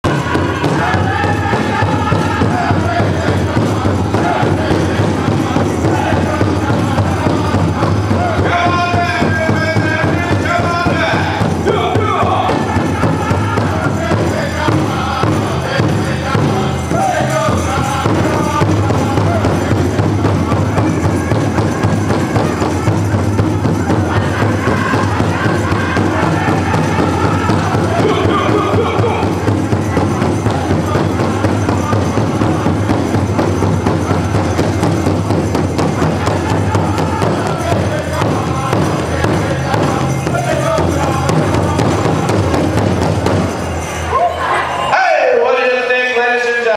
Men's Fancy Dance Special Sg. 3 in Thief River June 2017
Saturday Night Live Committee Specials at Seven Clans Thief River Powwow June 9-11, 2017 in Thief River Falls, Minnesota